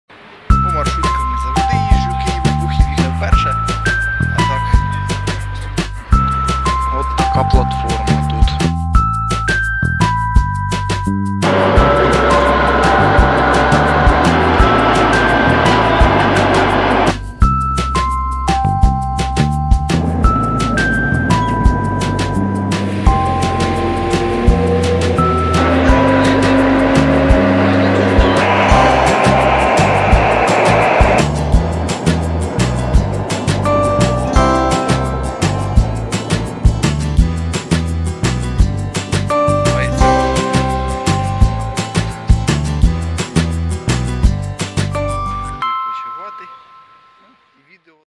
Поїздка на електричці у версії від Google Photos
Це твір від Google Photos на основі цього відео:
Як завжди цікаво глянути, на те, що в них робиться із завантаженнями.